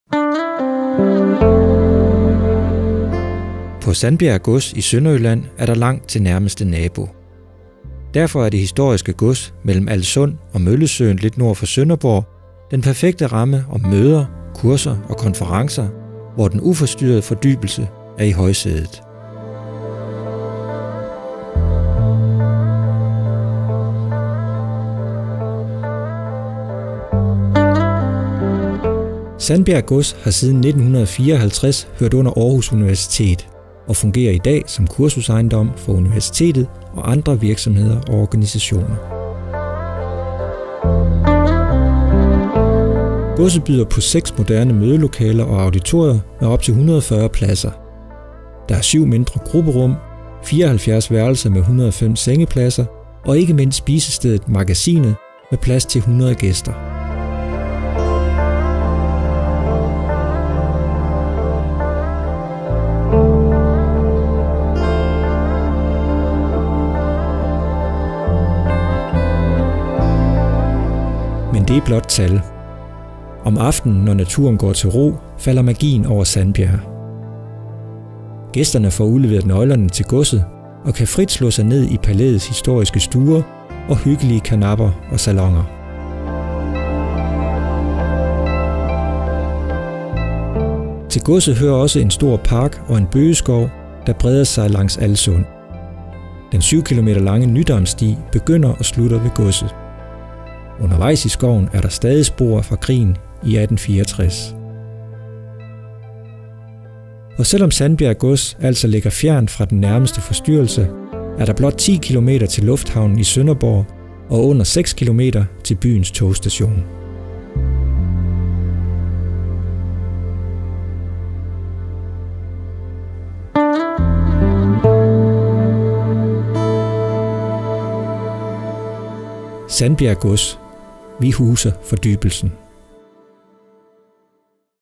Sandbjerg Gods - vi huser fordybelsen
På Aarhus Universitets kursusejendom, Sandbjerg Gods i Sønderjylland, er der langt til den nærmeste forstyrrelse. Derfor er det historiske gods mellem Alssund og Møllesøen lidt nord for Sønderborg den perfekte ramme om møder, kurser og konferencer, hvor den uforstyrrede fordybelse er i højsædet.